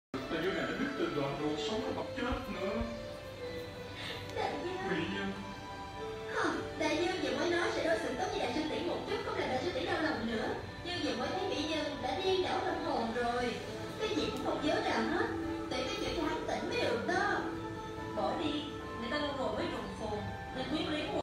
Sleepy Puppy Hugs Hamster Best Friend || ViralHog
Occurred on August 13, 2019 / Ho Chi Minh City, Vietnam

"These two little babies hug each other and sleep so cute. Friendship deserves respect."